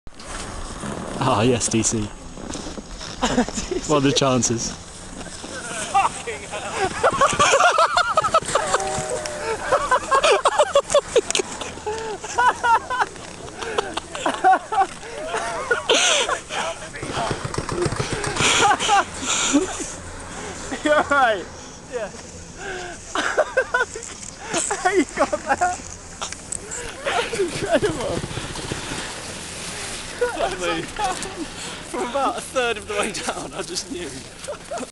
Ultimate Stack
DC outdoes himself attempting a tricky dog-leg jump at Pila, Italy 2006